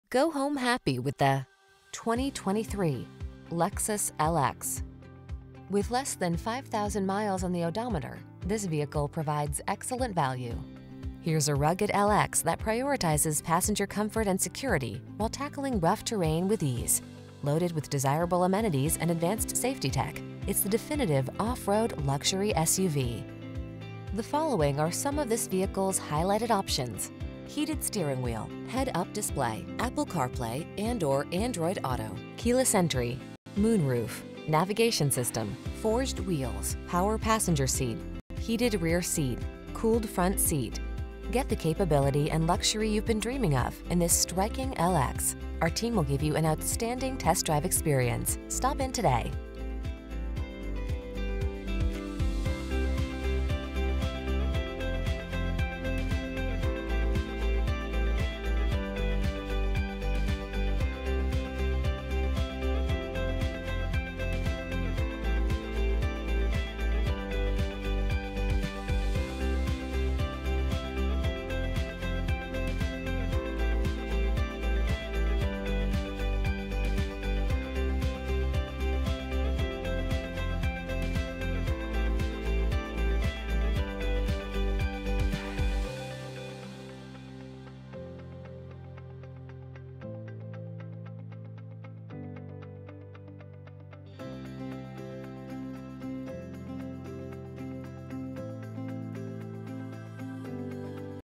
2023 Lexus LX 600 Luxury IL Chicago, Glenview, Palatine, DesPlaines, Evanston
Pre-Owned 2023 Lexus LX 600 Luxury available at Fields Lexus Glenview located in 2000 Waukegan Rd, Glenview, IL, 60025. Servicing the Chicago, Glenview, Palatine, DesPlaines, Evanston area.

Go home happy with the  2023  Lexus LX.  Here's a rugged LX that prioritizes passenger comfort and security while tackling rough terrain with ease. Loaded with desirable amenities and advanced safety tech, it's the definitive off-road luxury SUV. The following are some of this vehicles highlighted options: Heated Steering Wheel
, Head-Up Display, Apple Carplay®/Android Auto®, Keyless Entry, Moonroof, Navigation System, Forged Wheels, Power Passenger Seat, Heated Rear Seat(s), Cooled Front Seat(s). Get the capability and luxury you've been dreaming of in this striking LX. Our team will give you an outstanding test drive experience. Stop in today!